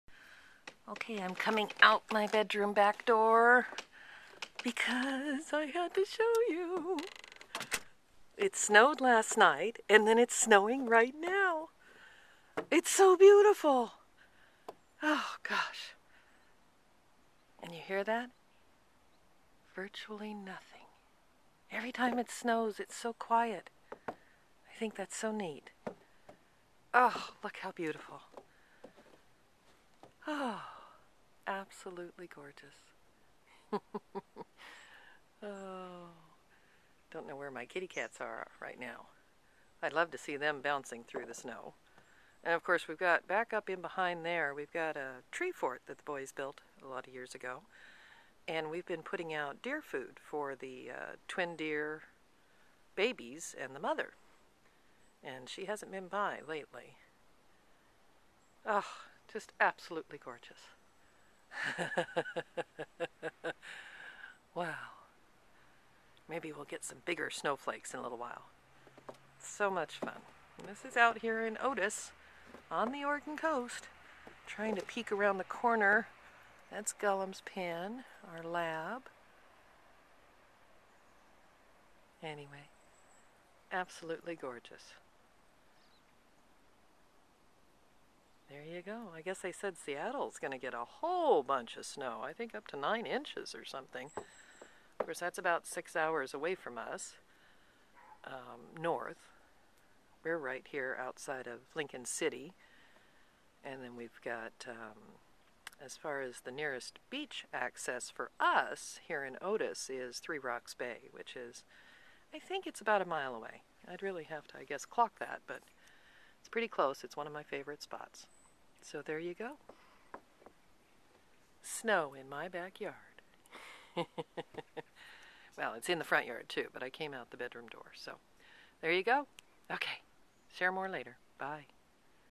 Snowing quiet clean pristine
So neat to experience snowing in my backyard, Otis Oregon Coast. It is so clean and quiet and pristine when it is snowing.